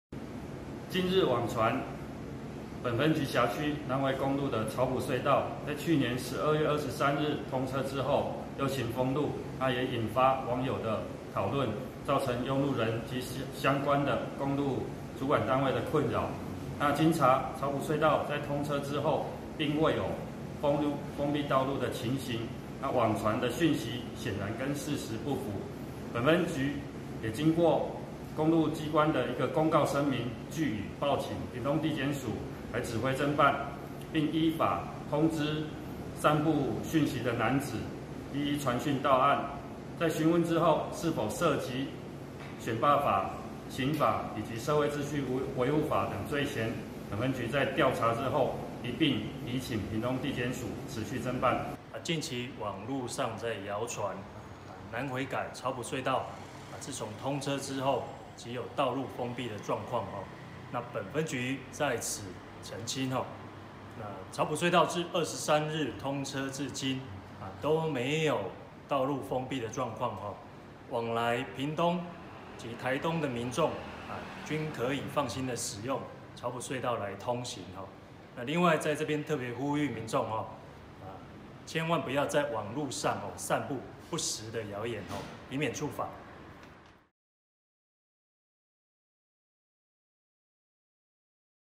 造謠南迴隧道「政治通車」又封閉 4人被函送(翻攝畫面)
省道台9線南迴公路改善工程，去年12月23日下午近5點正式開通橫跨雙東的長隧道，不過，有網友卻在當天開通之前，錄影並加上旁白，指不是已通車，為何不能開，影片再被網友「加料」，指稱只是為供行政院長蘇貞昌開吉普車驗收的「政治通車」後就又封閉，枋寮警方蒐證，日前已通知4人前來說明，並依相關罪嫌函送屏東地檢署偵辦。